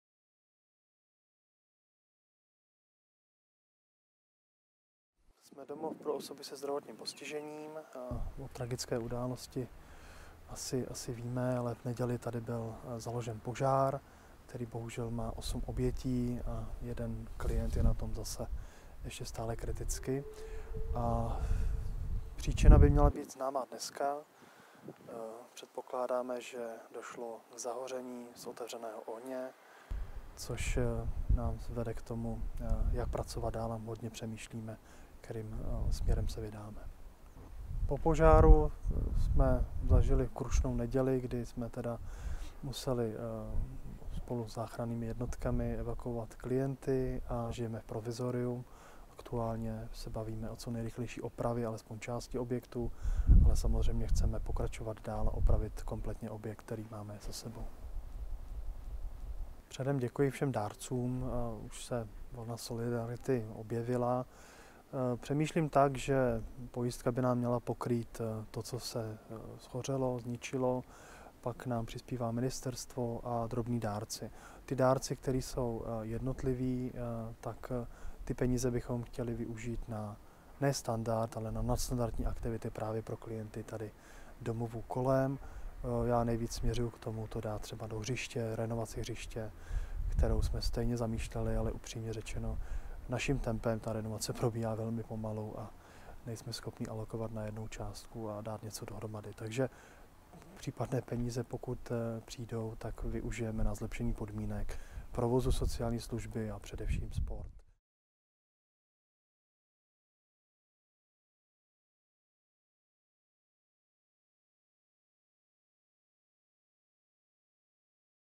Udělejme radost lidem, které postihl tragický požár ve Vejprtech
Udělejme radost lidem, které postihl tragický požár ve Vejprtech, při kterém zemřelo osm lidí a desítky byly zraněny.

Sami jsme domov navštívili a viděli, co za škody napáchal oheň na objektu, ale zejména jak tato tragická událost zamávala s životy lidí. Mnohem důležitější než čísla jsou pro nás totiž příběhy těch, pro které je zařízení jediným domovem a tamní zaměstnanci jsou jejich jedinou rodinou. Proto jsme se rozhodli že bychom klientům, kteří nyní žijí v provizoriu, rádi ukázali, že je čekají i hezké věci. A tak jim spolu s vámi pomůžeme upravit okolí jejich domova.